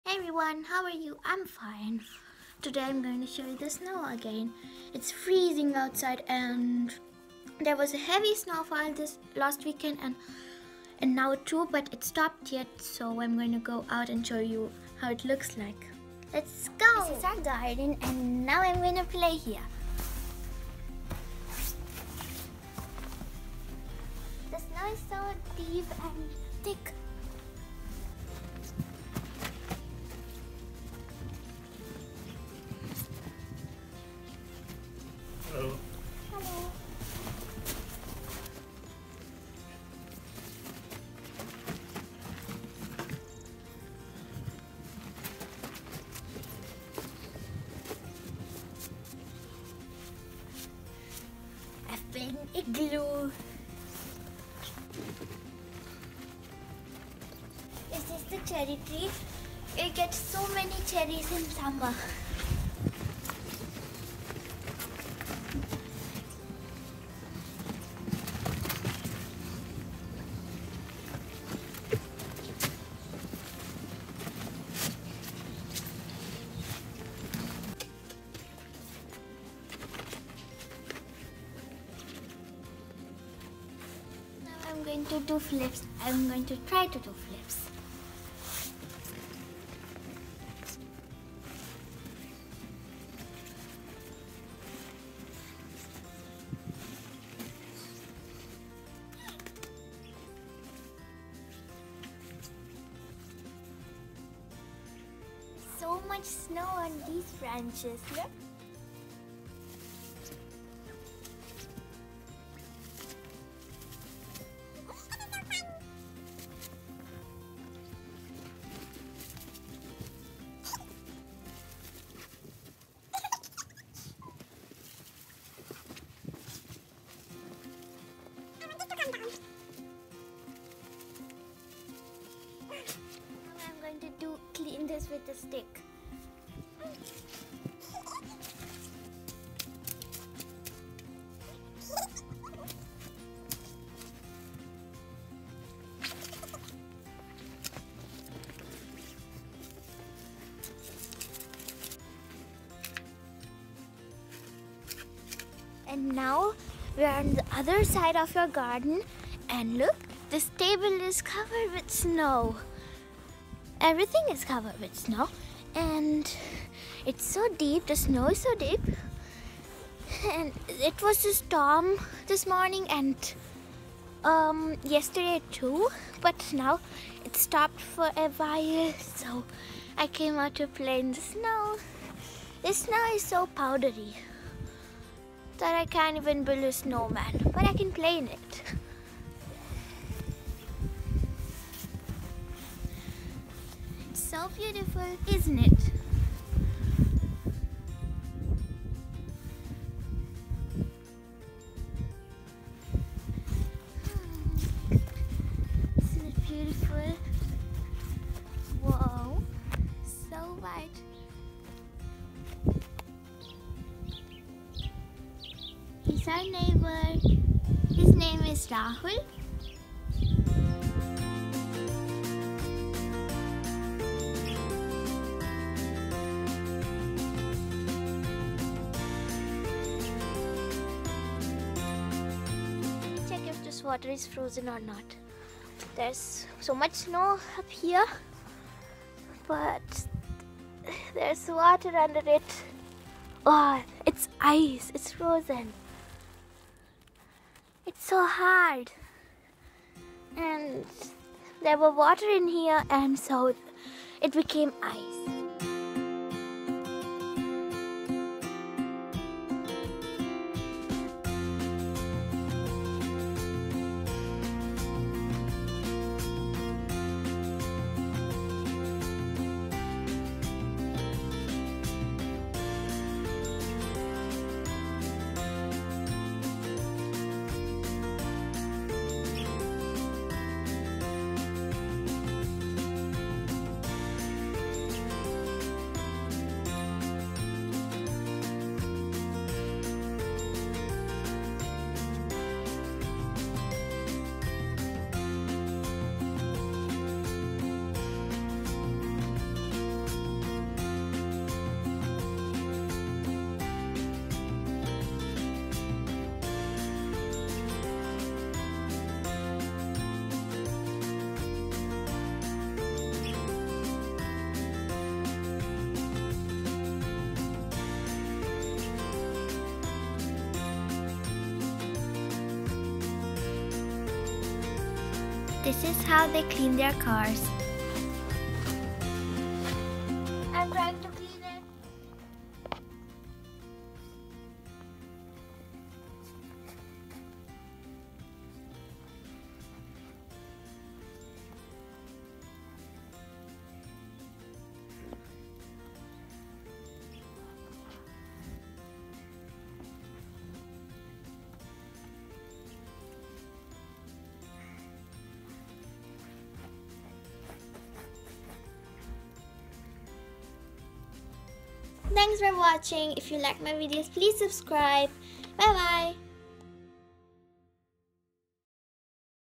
My surroundings in Germany after snow storm | మంచు తుఫాను తర్వాత మా ఇంటి దగ్గర పరిస్థితి
Heavy snow fall in Germany this week because of the snow storm. Everywhere it is filled and covered with snow. Have a look at the surroundings of my home and I played a lot in the snow.